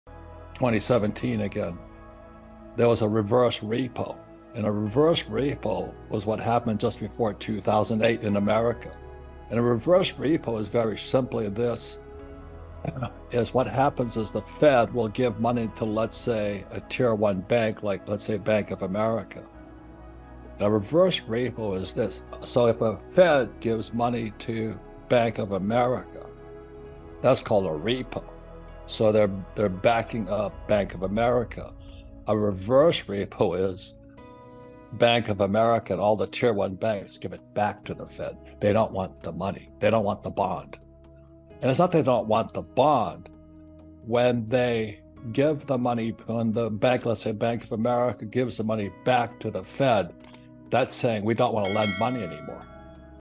Robert Kiyosaki: U.S Banks Are in Trouble? 😬🤔
Robert Kiyosaki: U.S Banks Are in Trouble? 😬🤔. This robert kiyosaki bank collapse news informs of how the U.S banks are in a big mess. Looking at the latest prediction of robert kiyosaki biggest crash about the economy. This video will  highlight more insight.